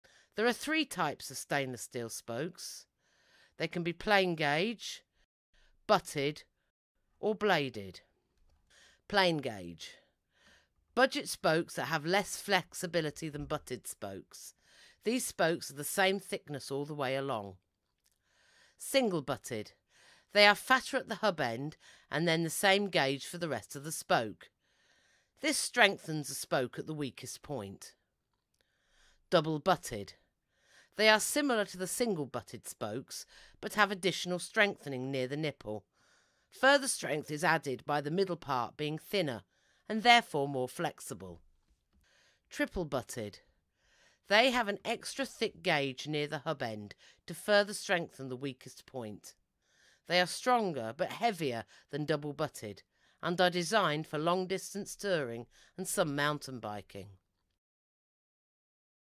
Types of spokes explained - Breaking down the different varieties
Different types of spokes explained by their strength.
In this informative short video, we're diving into the world of spokes, exploring the characteristics and advantages of Plain Gauge, Single Butted, Double Butted, and Triple Butted spokes.

🔧 Topics Covered in This Video:

🛠️ Plain Gauge Spokes: We'll start with the straightforward Plain Gauge spokes, explaining their design and where they excel in the cycling world.

🔩 Single Butted Spokes: Next, we'll explore Single Butted spokes and why they're favored for specific riding styles.

✌️ Double Butted Spokes: Then, we'll discuss Double Butted spokes and their unique benefits for various cycling disciplines.

🤙 Triple Butted Spokes: We'll finish up with Triple Butted spokes, uncovering their advanced design and how they impact high-performance biking.

💡 Advantages and Use: Throughout the video, we'll highlight the advantages of each spoke type and where they shine.

Oh, and don't forget about our FREE Online Basic Maintenance Course. You can find it here:

00:00 - 3 types of stainless steel wheel spokes
00:08 - Plain gauge spokes
00:18 - Single butted spokes
00:28 - Double butted spokes
00:42 - Triple butted spokes